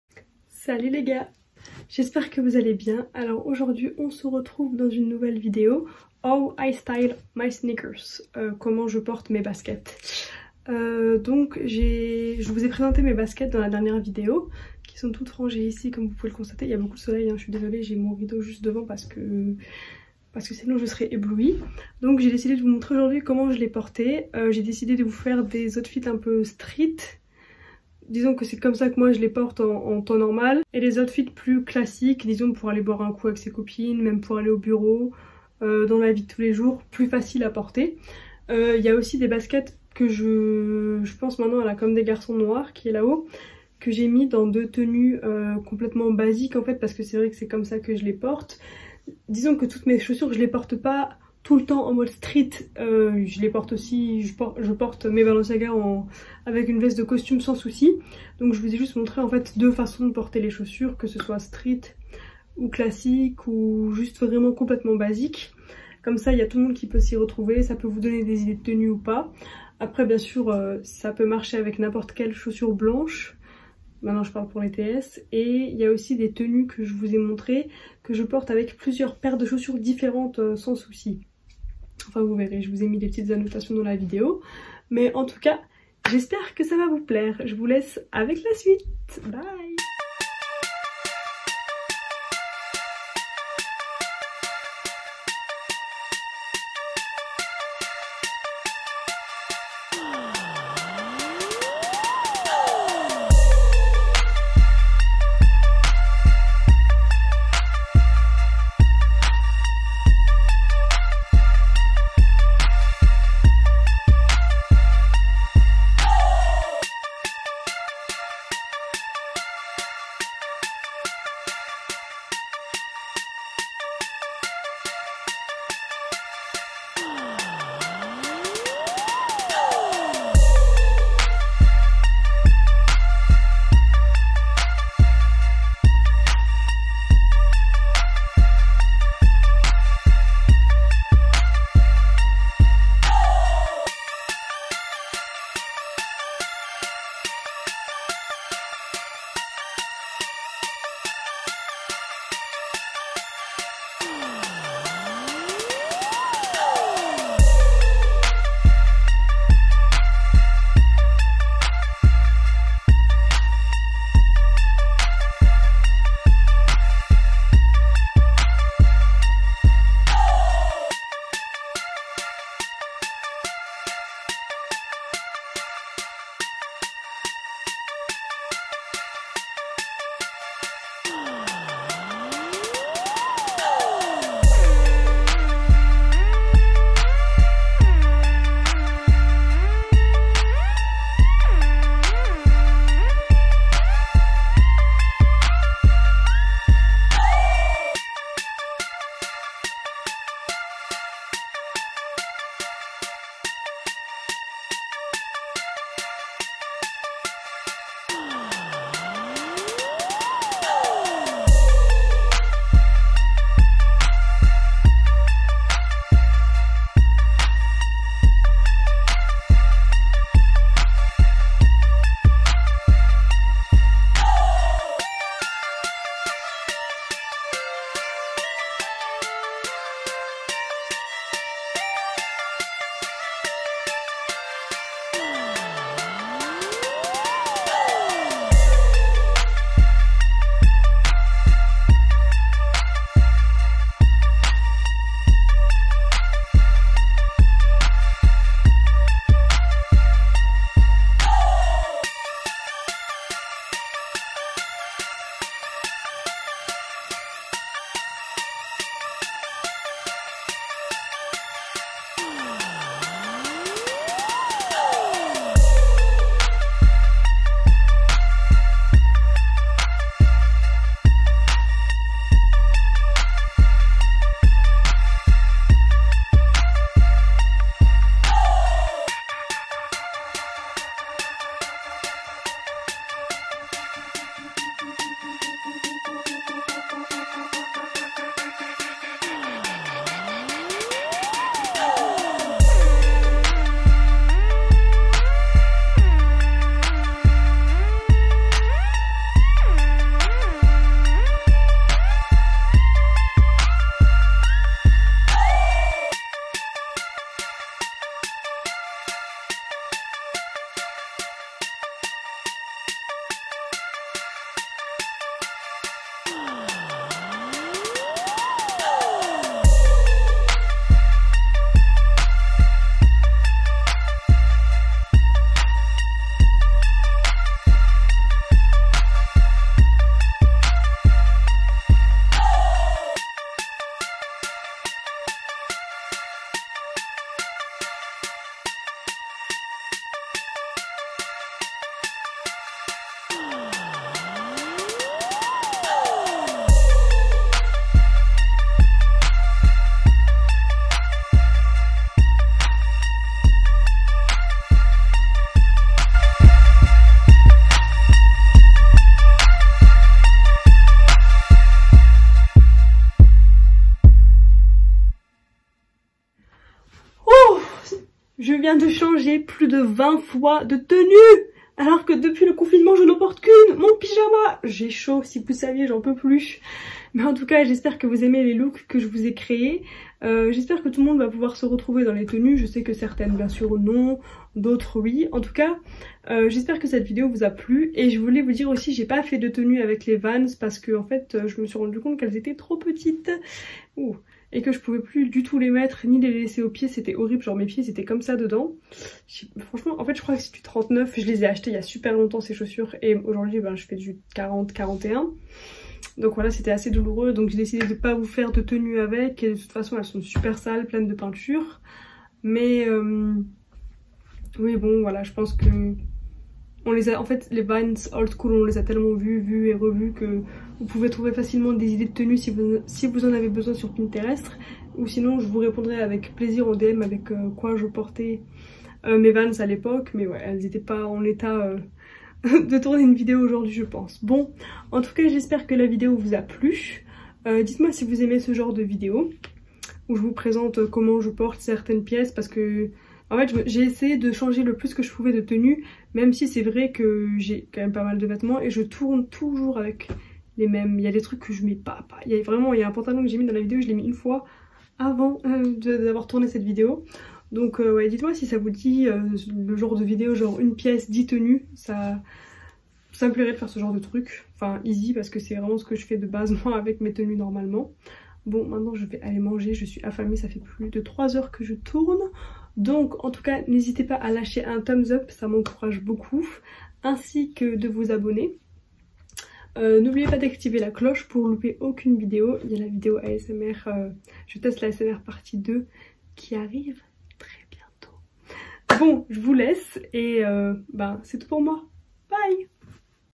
HOW I STYLE MY SNEAKERS (17 outfits) vidéo en Français
Hello les amis aujourd’hui on se retrouve dans une nouvelle video ou je vous montre comment je porte mes sneakers bon Visionnage,

OUTFIT 4:

dress: disponible sur mon vinted

OUTFIT 5:

cycliste:  dispo sur mon vinted

OUTFIT 12:

OUTFIT 13:

le top dispo sur mon vinted

Je suis désolé mais énormément de liens sont des dupes Il faut savoir que j’achète énormément de mes vêtements lors de mes voyages car les vêtements en France ne me conviennent pas les boutiques sont souvent des boutiques indépendantes qui n’ont pas de site Internet il y a aussi énormément des vêtements que je vous ai présenté que j’ai trouvé en friperie ￼￼￼

love u ❤️